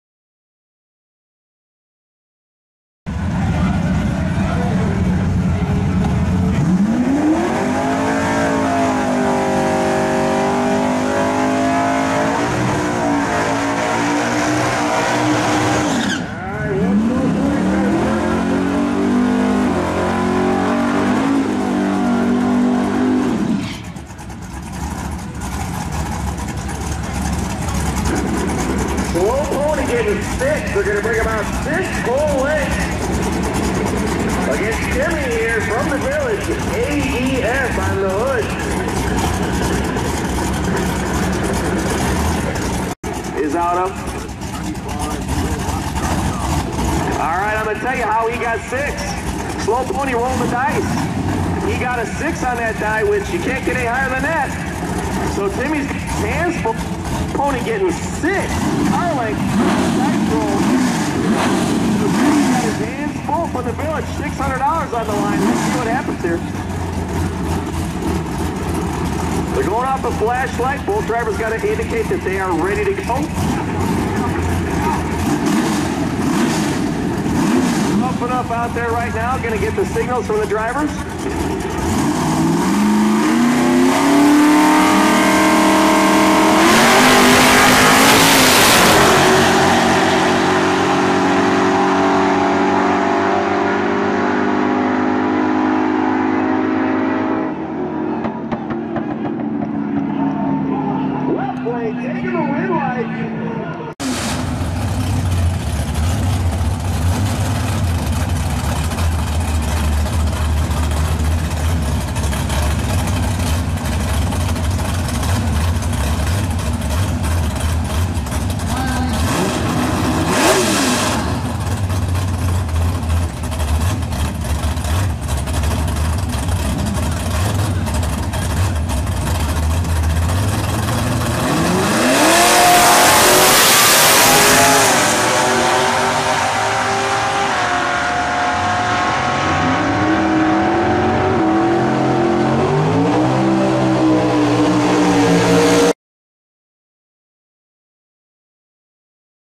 Chris Henderson's "Slow Pony" Mustang / Archive Footage
Chris Henderson's "Slow Pony" making a couple passes at GLD